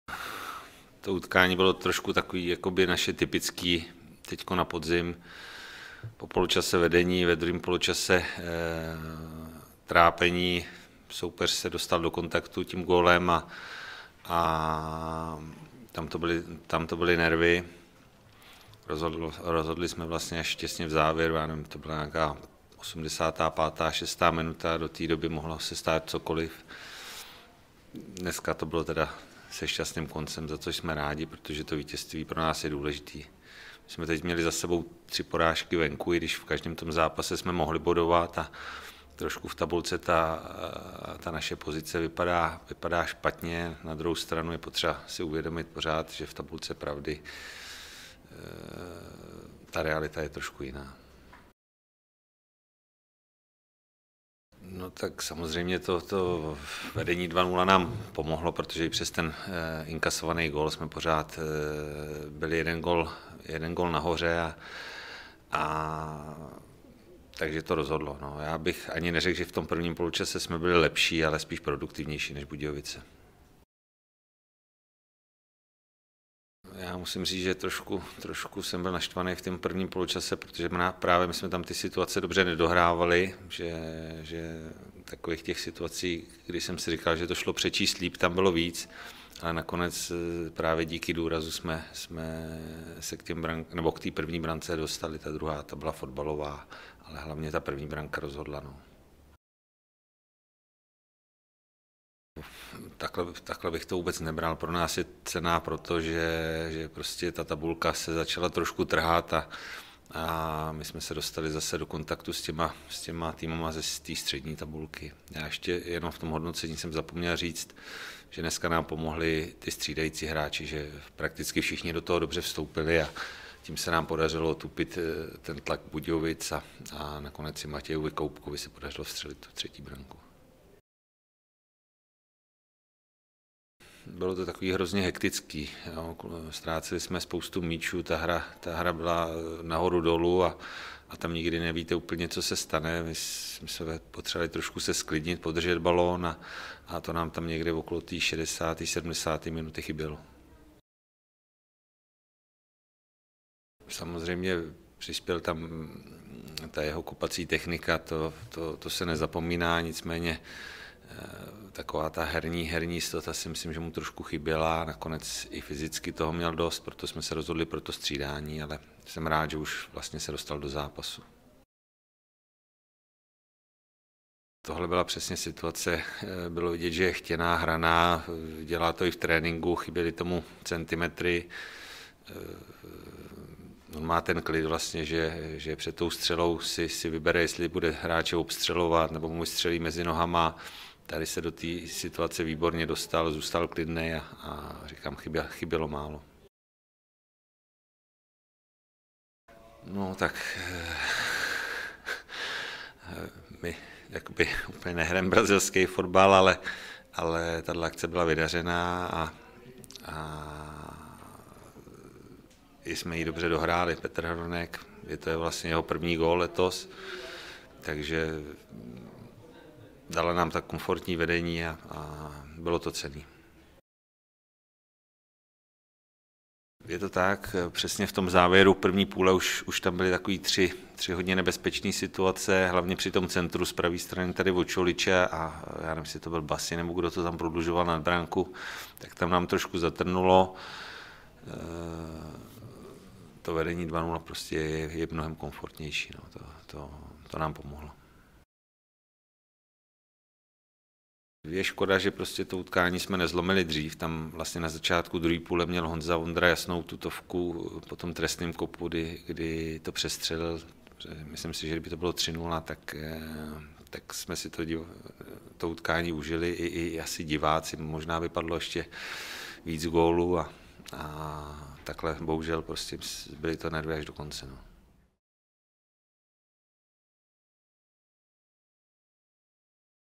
TISKOVKA BOHCEB | Jubilejní 100. zápas trenéra Klusáčka Bohemka vyhrála
Klokany dnes vedl do 80. zápasu v lize. Dalších 20 jich přidal v Jihlavě. Góly Keity, Hronka a Koubka znamenaly vítězství nad Dynamem ČB 3:1. Utkání na tiskovce hodnotil hlavní trenér Luděk Klusáček.